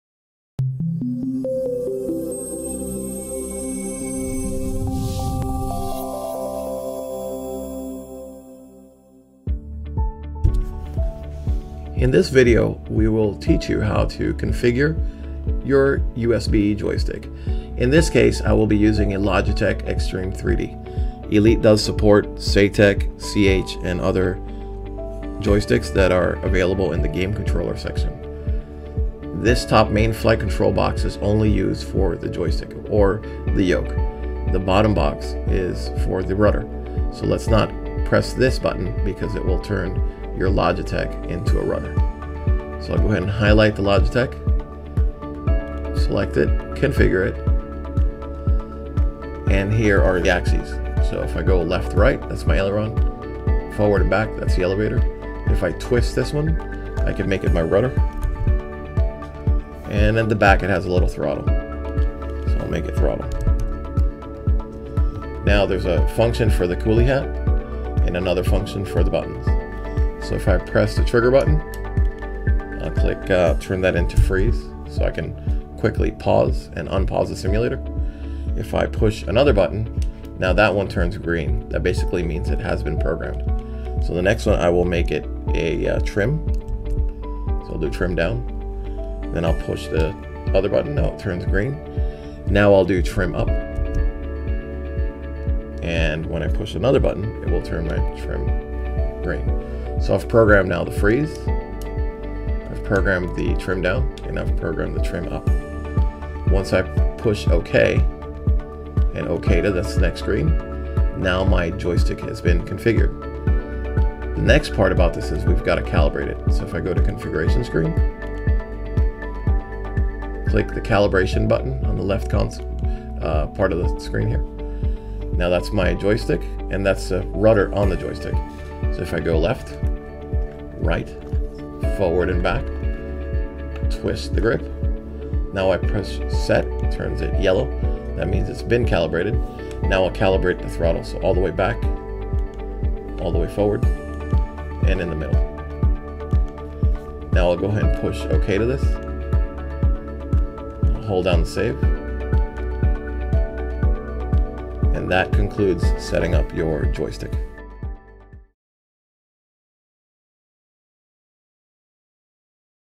How to configure the joystick or yoke in ELITE XTS?
This video illustrates how to configure the joystick or yoke in  ELITE v9 XTS.